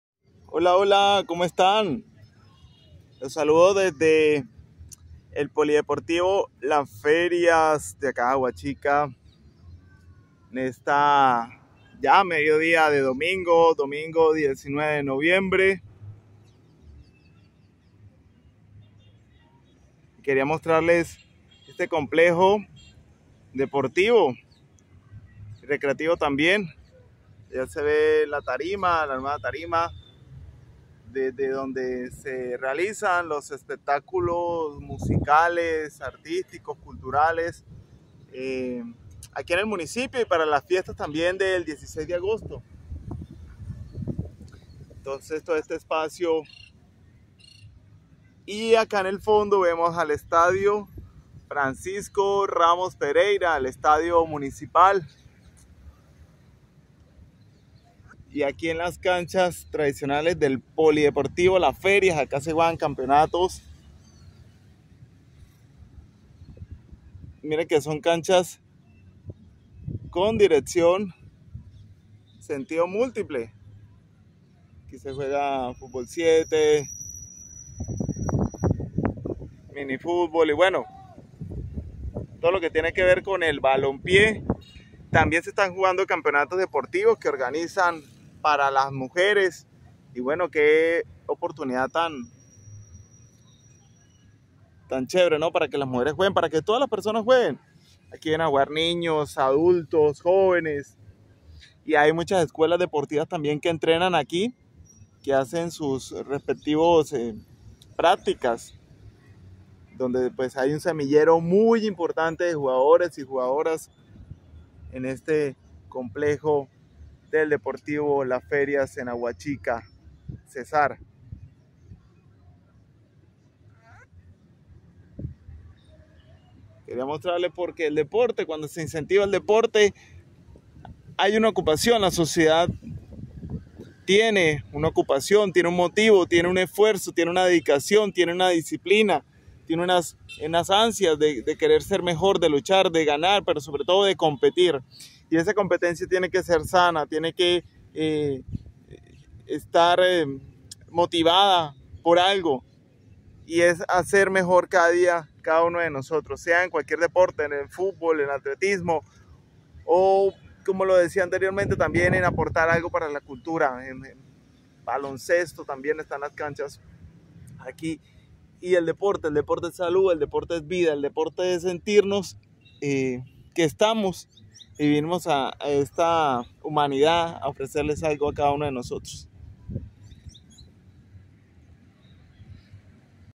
El deporte y la recreación, fundamentales en la sociedad
Recorriendo en bici los escenarios de Aguachica